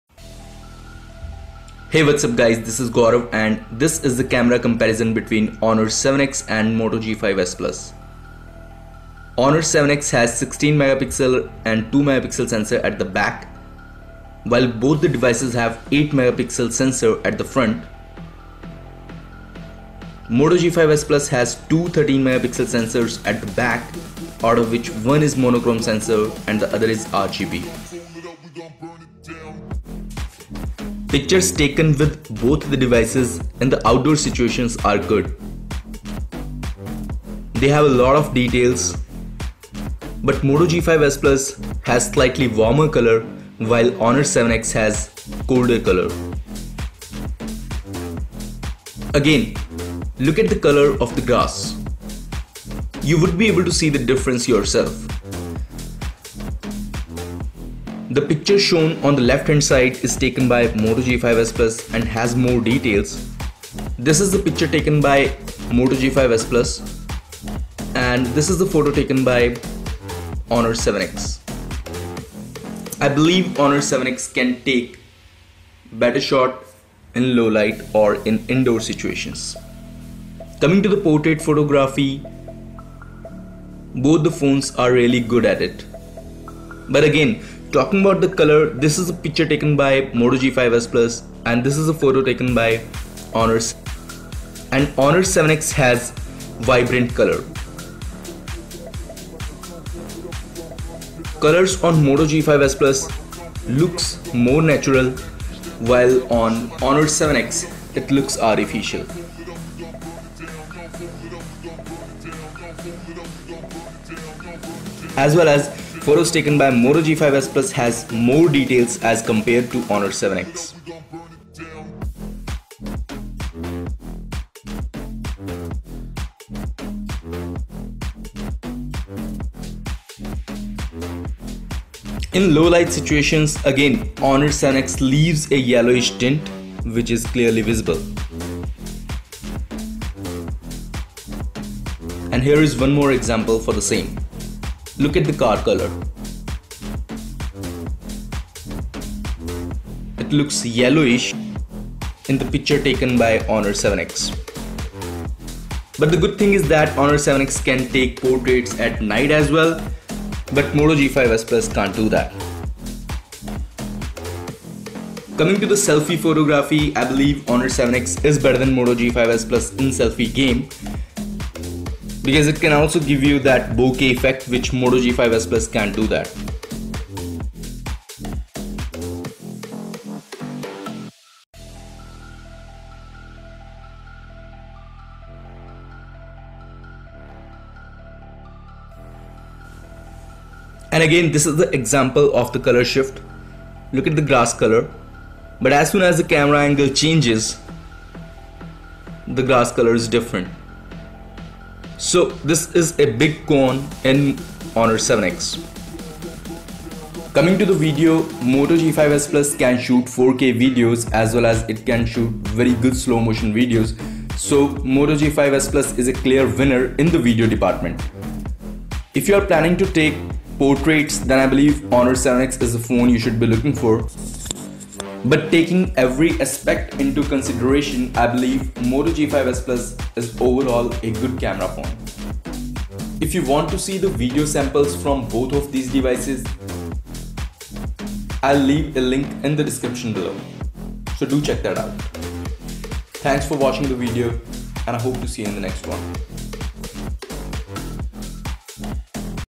Honor 7X vs Moto G5S Plus Dual Camera Comparison
Hi Guys, This is Gaurav and This is the Camera Comparison between Honor 7X and Moto G5s Plus.

Cheap DSLR for YouTube

Honor 7X Video Sample:-
Moto G5s Plus Video Sample:-

Honor 7X takes better photos in terms of bokeh effect which works at night as well. But in terms of color game, Moto G5S plus captures the better and natural colors.

Downside is Honor 7X can't shoot Video at 4K and Slow Motion video at 640p is just not usable while Moto G5s plus shoots 720p slo-mo video which is much better than Honor 7X.